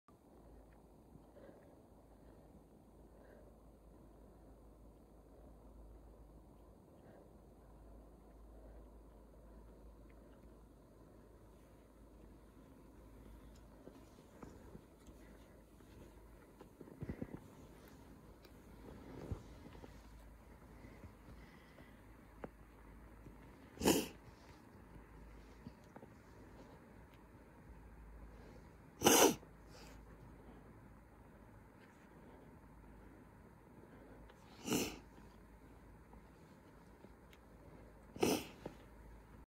地形でB360したつもりが全くジャンプになってなかったスノーボーダー
🎥：どくりんご